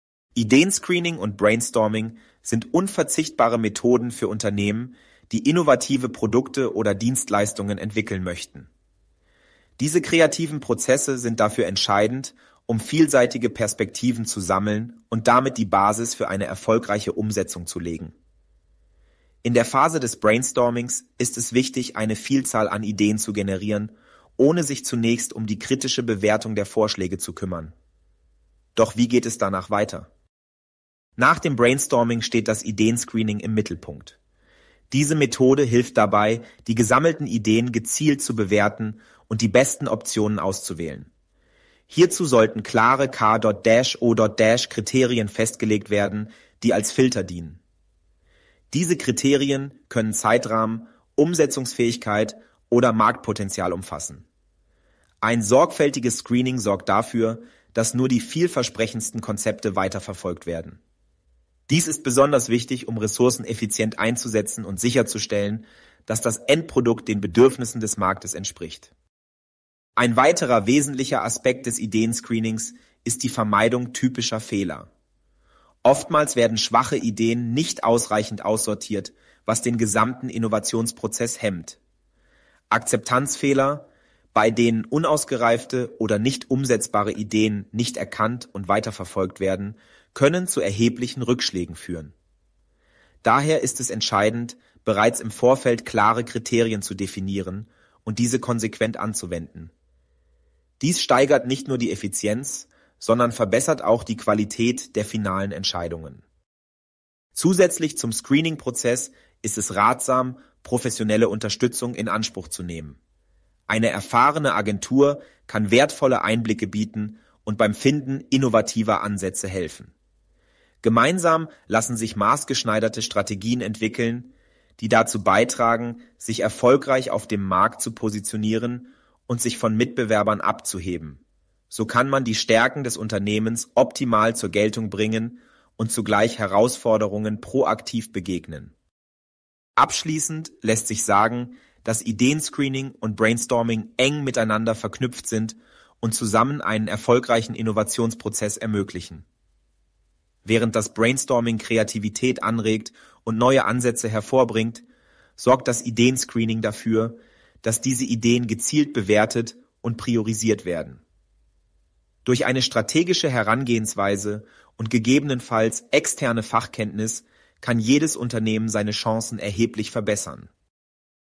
IDEA SCREENING warum es so wichtig ist
n diesem Video erfährst du alles über das Thema IDEA SCREENING und warum es für den Erfolg deiner Projekte unerlässlich ist. Wir zeigen dir, wie du deine Ideen effektiv bewerten und auswählen kannst, um sicherzustellen, dass du deine Ressourcen optimal nutzt.
Egal, ob du ein Unternehmer, ein kreativer Kopf oder einfach nur jemand bist, der seine Ideen zum Blühen bringen möchte – dieses Video richtet sich an dich! Wir erklären die Schritte des IDEA SCREENING-Prozesses und geben dir wertvolle Tipps, wie du potenzielle Herausforderungen frühzeitig erkennen und vermeiden kannst.
Lass uns gemeinsam herausfinden, wie du deine Ideen zum Leben erwecken kannst! Teile auch gerne deine Gedanken in den Kommentaren!